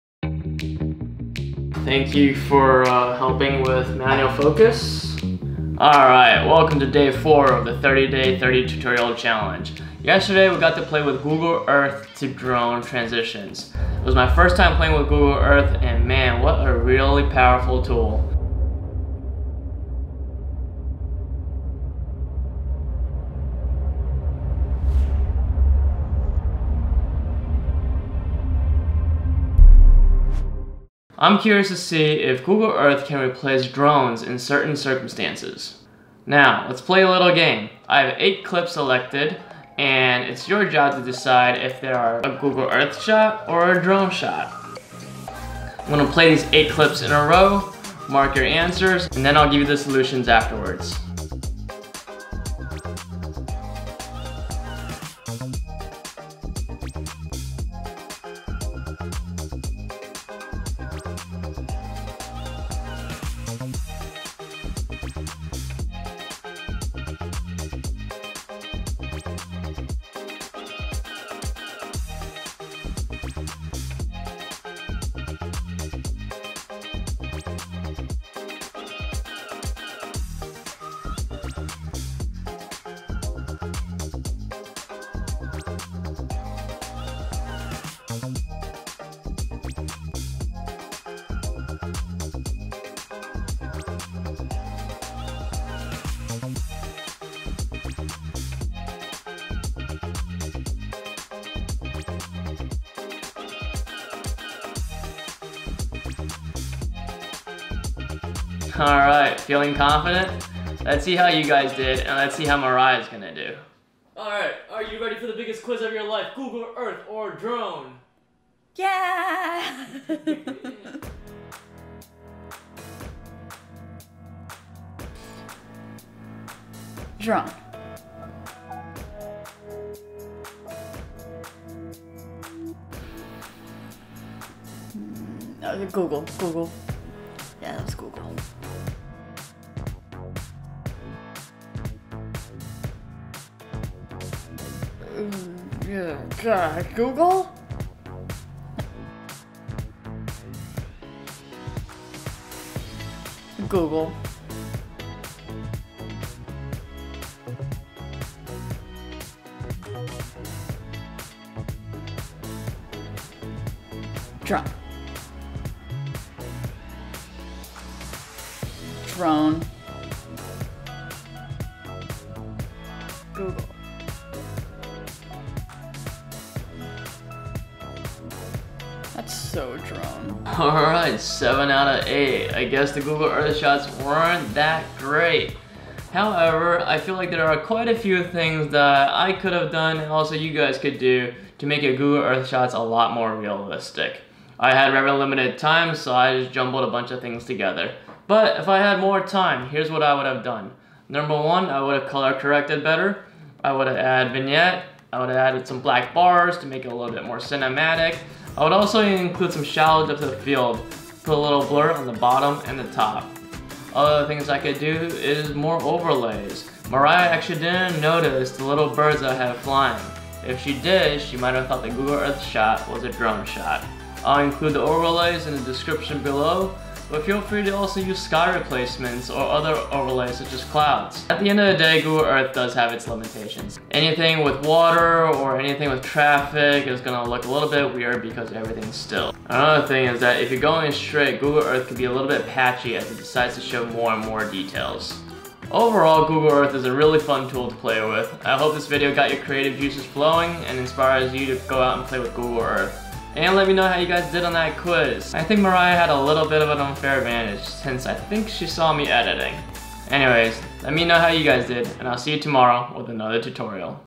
Google Earth or Drone? Can YOU tell the difference?! + Tutorial!
How many did you score?!
Also, here are some hotkeys for Google Earth:
Hold ALT + Arrow Key for slow-mo
Hold ALT + SHIFT + Arrow Key  to rotate around a point in slow-mo

Also you can get real cool Pan & Tilt effect with a video game controller. See this guy's tutorial:

For bird overlays, just search "bird overlays" or "bird alpha channel" on YT. In premiere, use a color key effect and match the color with the background to get rid of it.

Day 4 of the teach2learn 30 day 30 tutorial challenge is done! Let's see if we can rack this video up to 50 whopping views!

Let's collab and connect!!